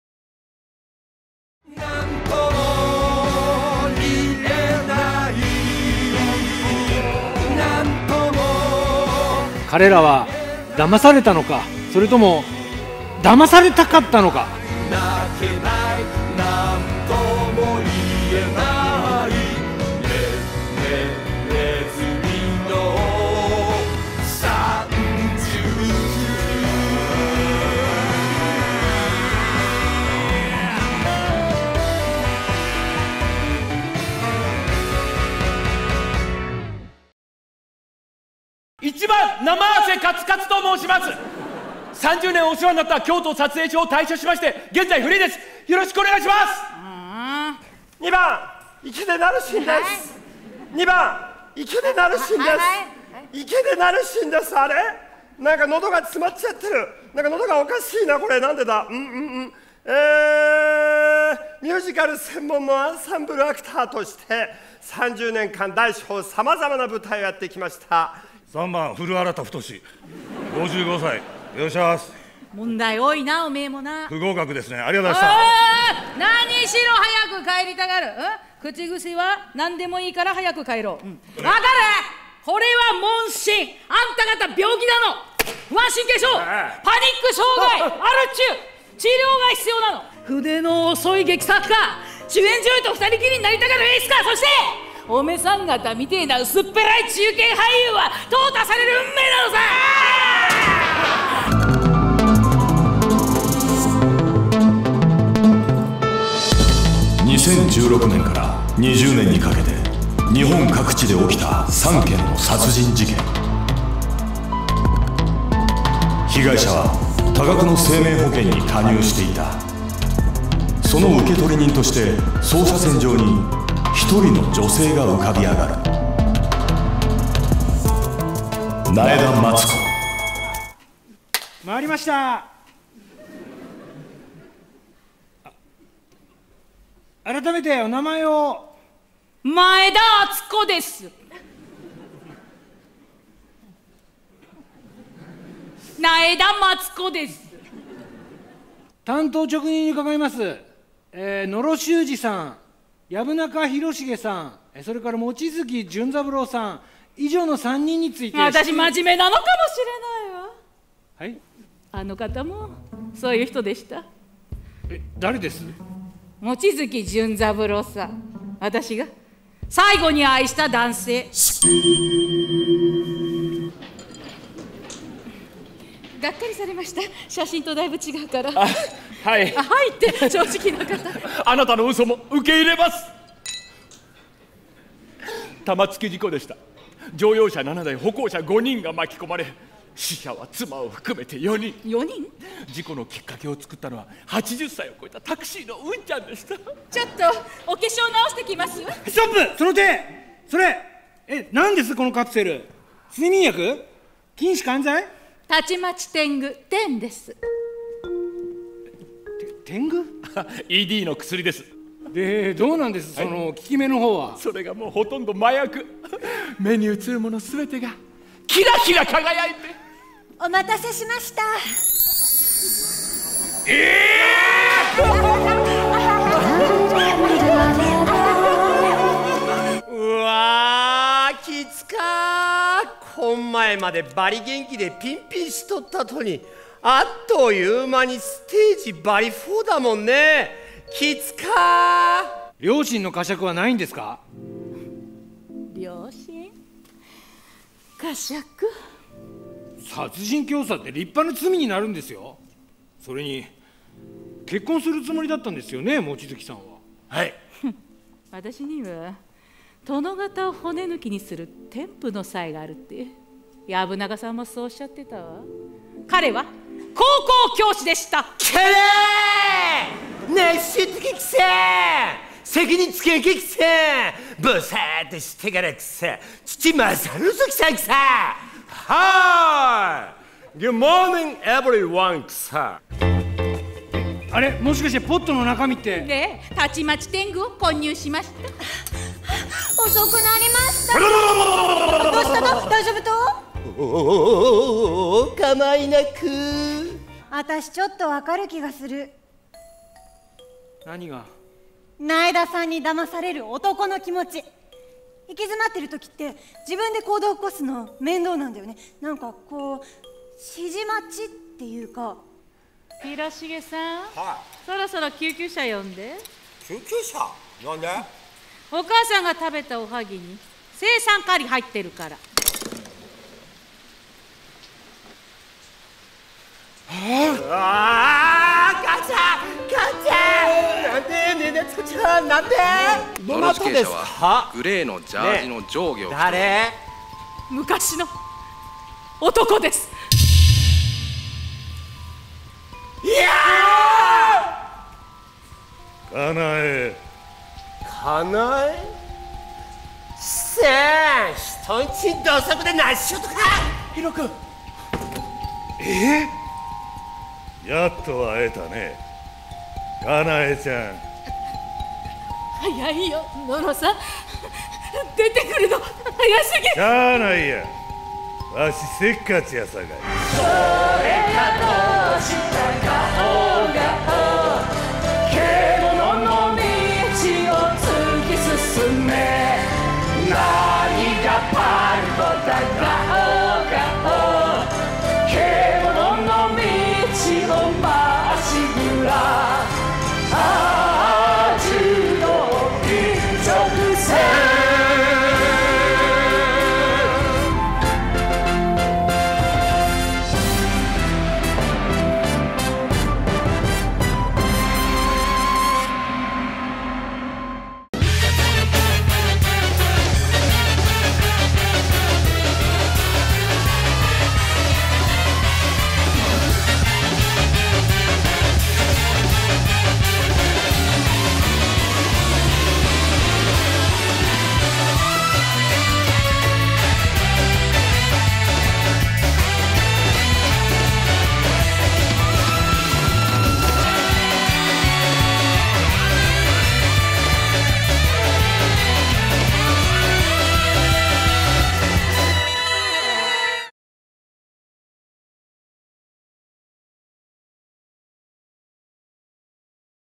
”JU-DO ICCHOKUSEN” PARCO Production 2020 Three Mouseketeers Vol.4 in Kyoto
PARCO Production 2020 Three Mouseketeers Vol.4
JU-DO ICCHOKUSEN
Kyoto / ROHM Theatre Kyoto: Thursday, Nov. 19. - Monday, Nov. 23.

The unit consists of the leading theater actors Katsuhisa Namase, Narushi Ikeda, and Arata Furuta.
Written by Kankuro Kudo, directed by Masahiko Kawahara, this is the latest title in their hit theater series with different guest stars.

Story
A series of murders shook the entire nation from 2016 to 2020.
The victims were all signed up for a match-making app, and also taking ED treatment. And the investigation led to a woman, Matsuko Naeda.
After a post regarding the woman and her involvement in the murders goes viral, a documentary writer Seki approaches her in an attempt to bring the truth to light.
Not naturally the most beautiful woman, how did she lure the victims?
Seki first looked down on the victims, but he starts to be drawn into the web of disturbing cases.

Bookwriter　Kankuro Kudo
Director　Masahiko Kawahara
Cast　　　Katsuhisa Namase    Narushi Ikeda   Arata Furuta
　　　　   Mizuki Yamamoto   Nobue Iketani   Kankuro Kudo

Planed by Nezumi no Sanjushi
Produced by PARCO Co.,Ltd.
Organized by SUNRISE PROMOTION OSAKA

”ねずみの三銃士”第4回企画公演
獣道一直線!!!
日時：2020年11月19日～11月23日
会場：ロームシアター京都メインホール

生瀬勝久、池田成志、古田新太による演劇界最強ユニット。
宮藤官九郎が新作を書き下ろし、河原雅彦が演出、毎回ゲスト俳優を迎え上演する人気シリーズ最新作！
Story
2016年から2020年にかけて、日本各地で起きた殺人事件。
被害者の共通点は、婚活アプリに登録していて、同じメーカーのED治療薬を飲んでいた。
そして、捜査線上に一人の女性が浮かび上がる。
「苗田松子」。
苗田松子が複数の殺人事件に関与しているという書き込みがSNSで拡散される中、ドキュメンタリー作家の関は、その噂を確かめるべく彼女に取材を申し込んだ。
決して美人でもないこの女になぜ男たちは騙されたのか。
はじめは被害者たちを滑稽に思う関だったが、自身も事件の闇に取り込まれていくのだった。

作：宮藤官九郎　演出：河原雅彦
出演：生瀬勝久　池田成志　古田新太
　　　山本美月　池谷のぶえ　宮藤官九郎
企画：ねずみの三銃士　製作：株式会社パルコ
主催：サンライズプロモーション大阪